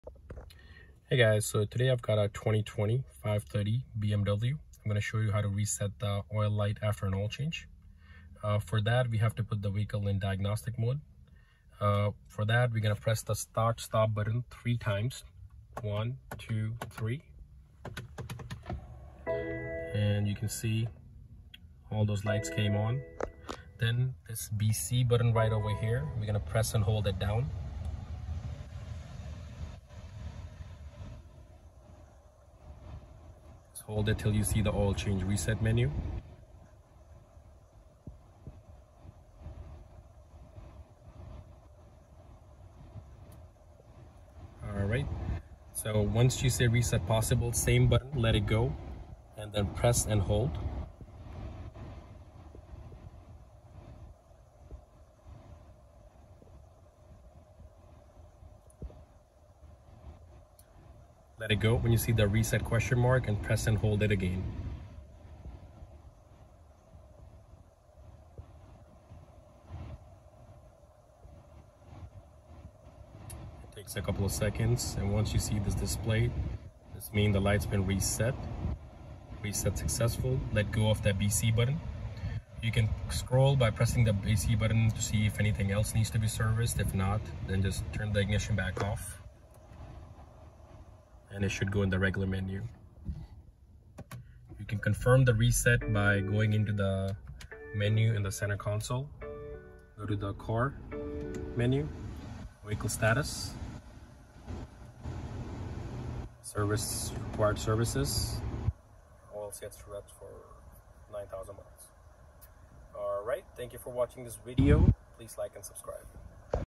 2020 BMW 530 oil reset light maintenance reset procedure
The video shows the reset procedure for oil maintenance light for 2020 BMW 530.